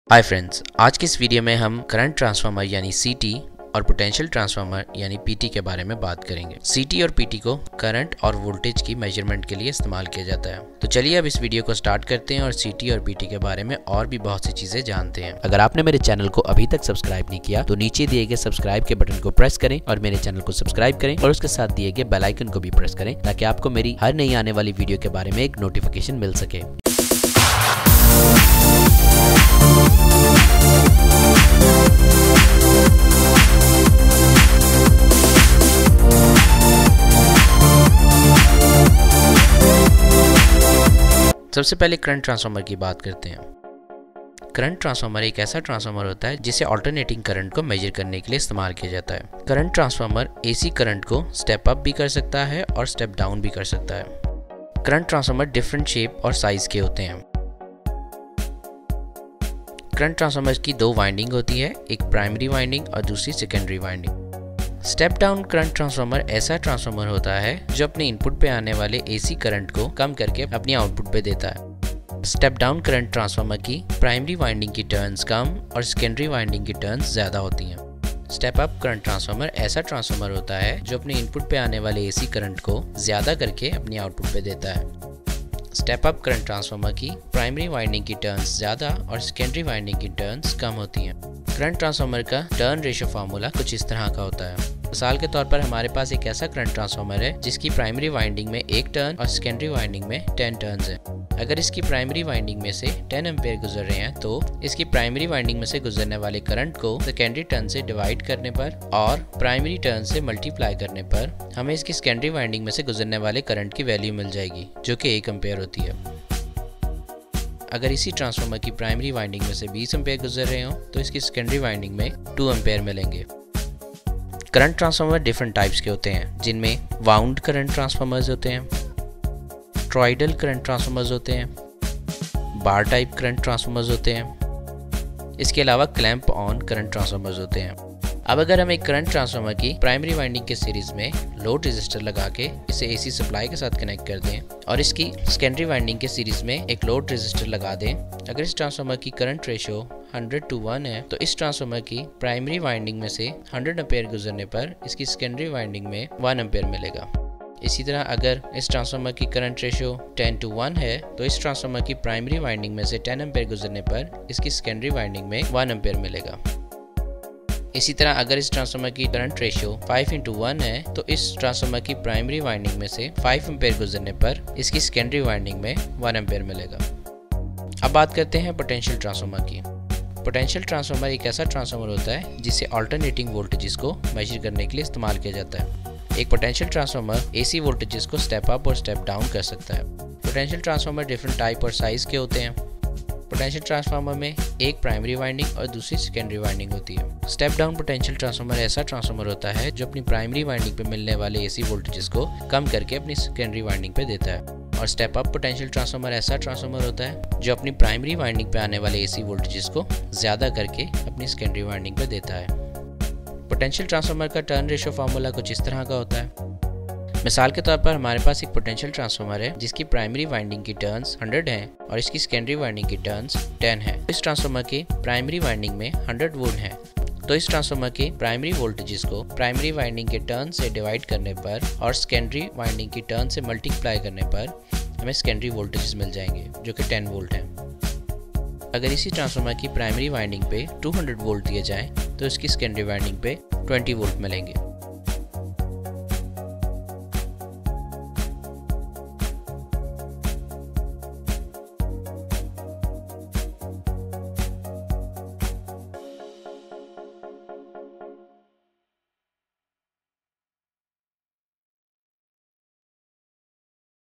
What is CT and PT
hi friends in this video i have talked about current transformer and potential transformer their types their turn ration formula hope you like this video please like share and subscribe my channel